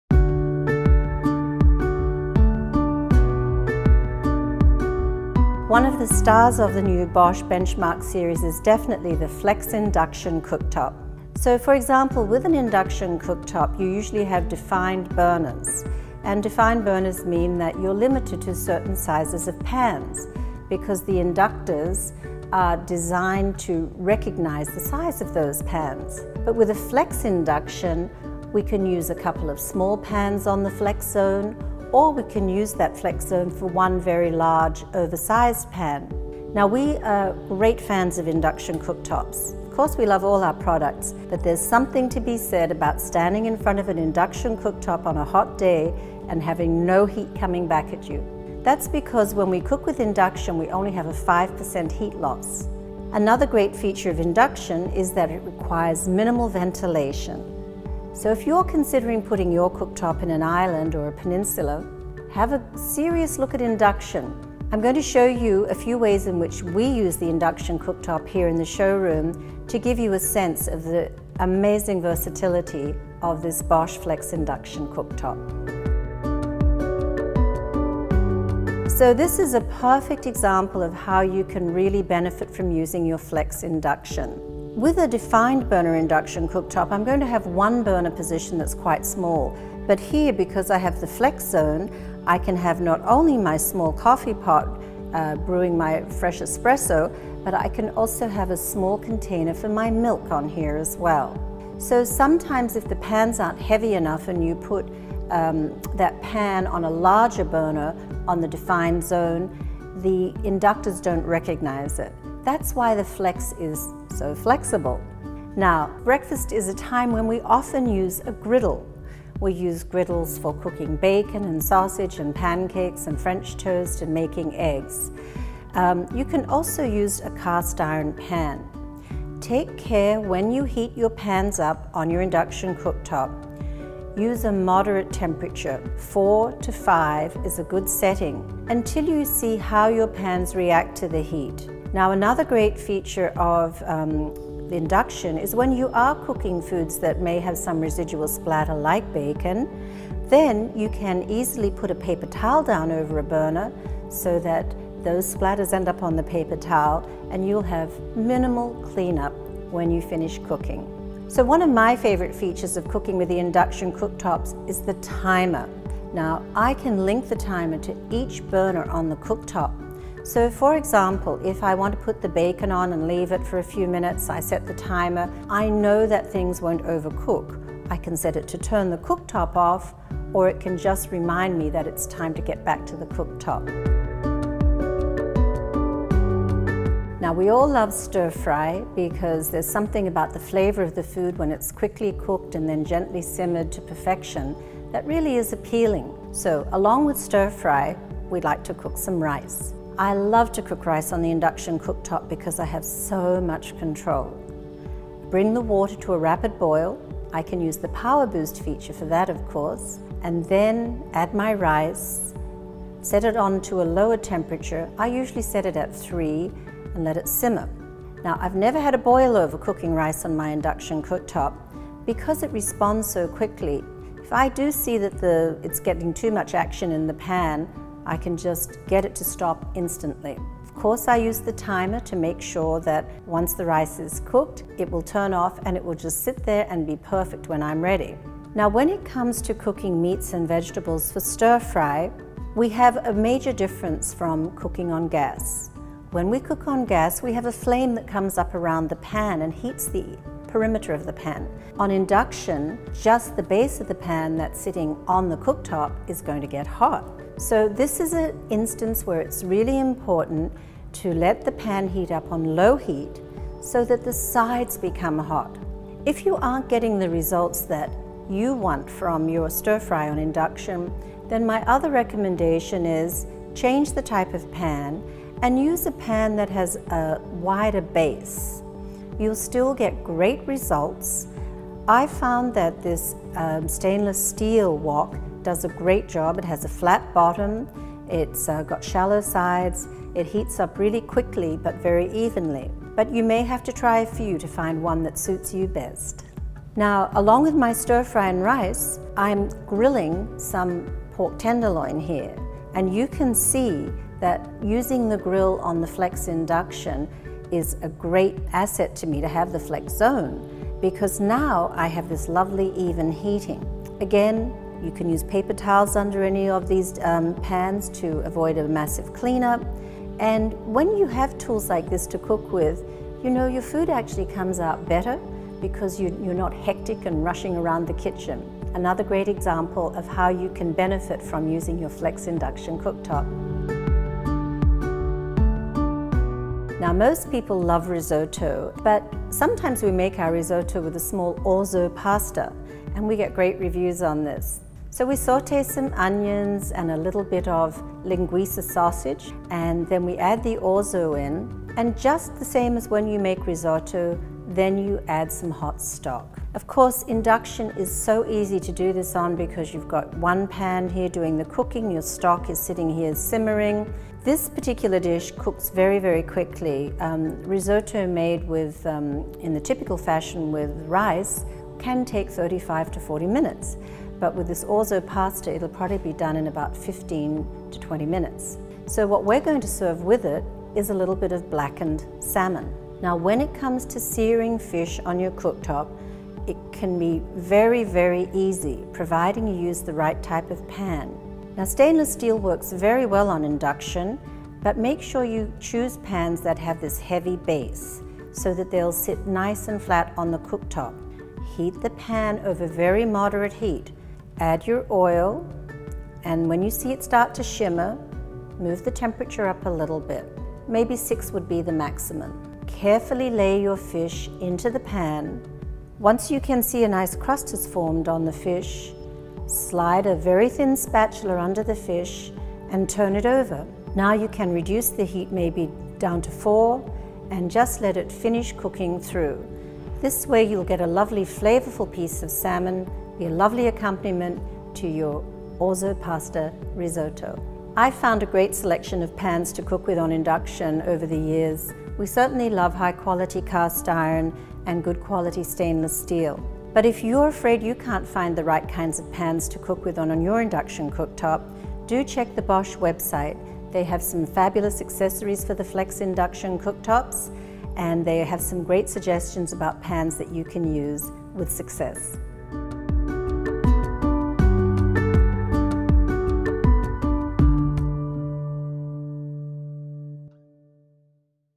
Bosch Flex Induction Cooktop
Bosch Flex Induction Cooktops feature an expanded zone with sensors that enables you to cook with large griddles, odd-shaped pans or very small pans.The expanded zone can also be used as two independant burners.